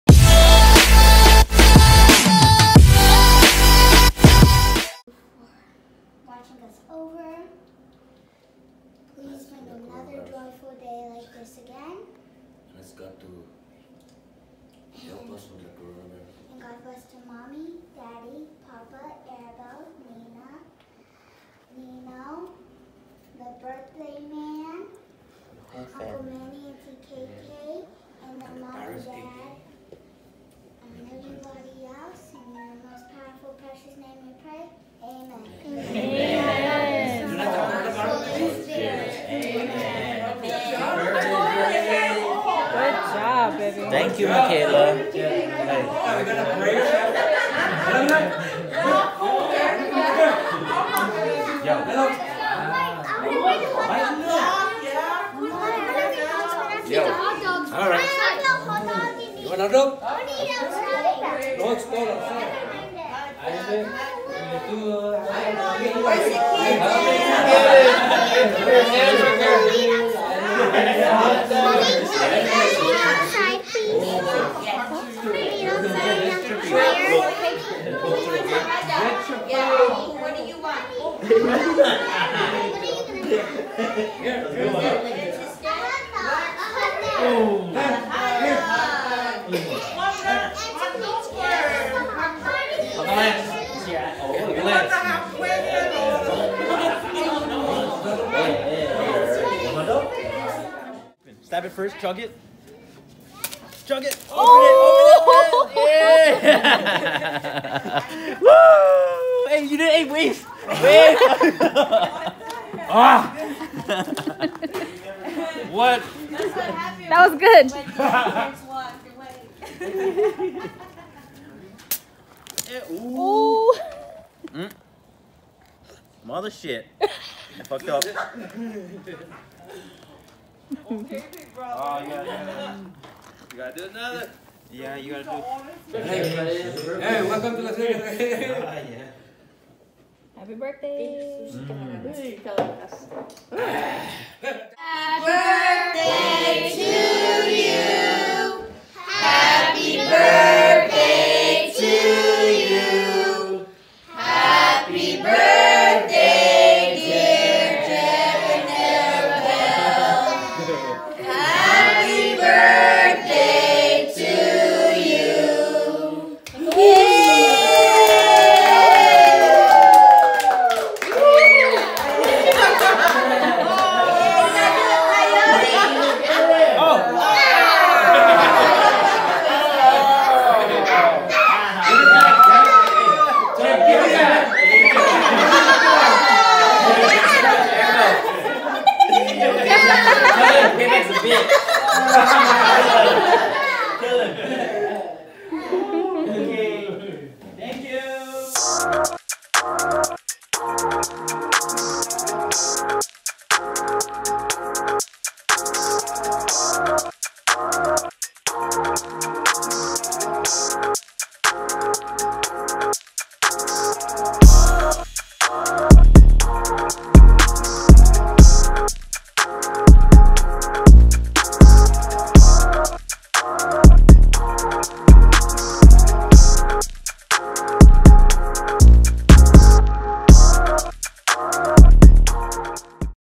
Happy Birthday Jeb!
Birthday party for Jeb at Michael and Nacia's house! A small family get together to celebrate. Burgers, hotdogs, beer, and cake!

Music at 3:18
Musician: Jeff Kaale